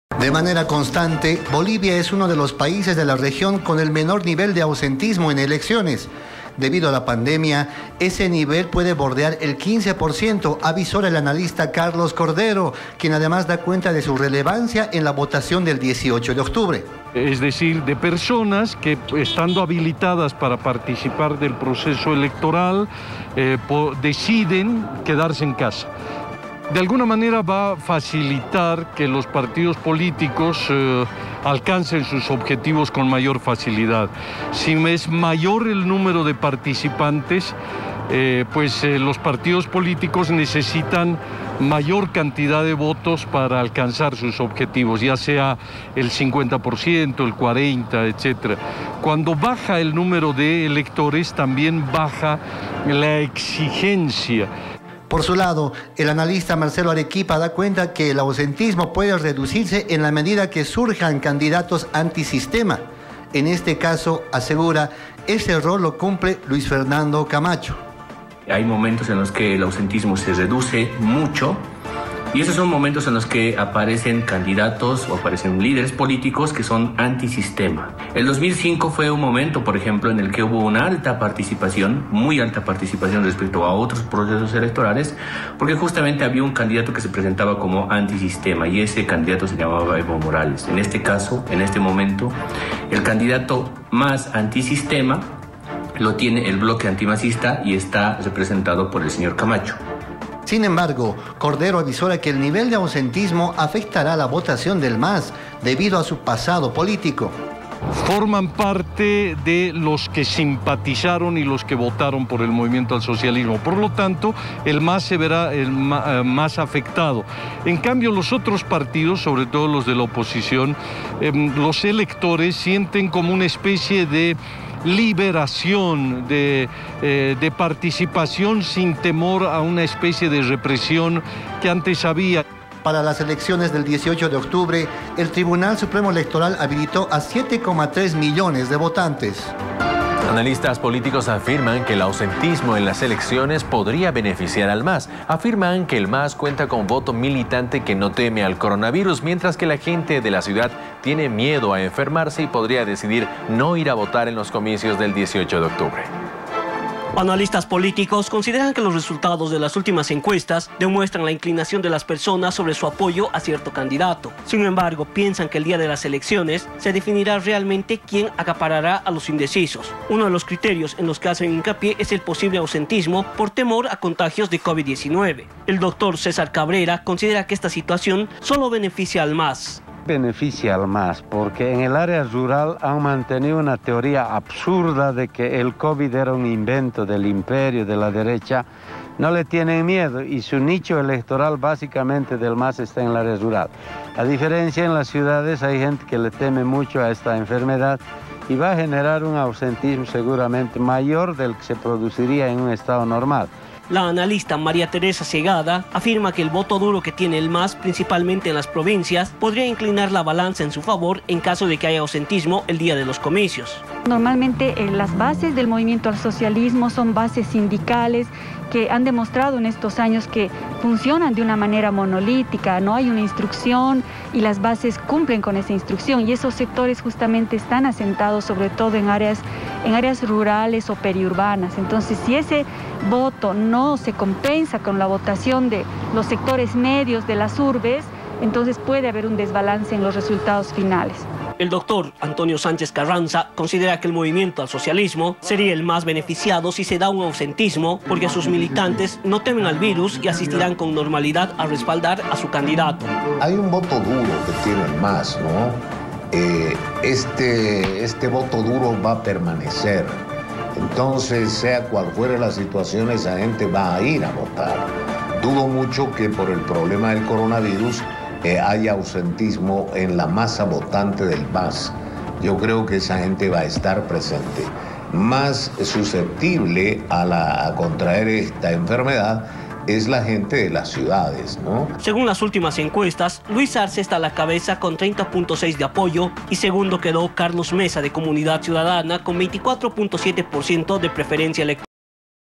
UNITEL 20201001 Ausentismo puede influir en la votación
Analistas políticos dan diferentes interpretaciones sobre el posible ausentismo ante temor por el Covid 19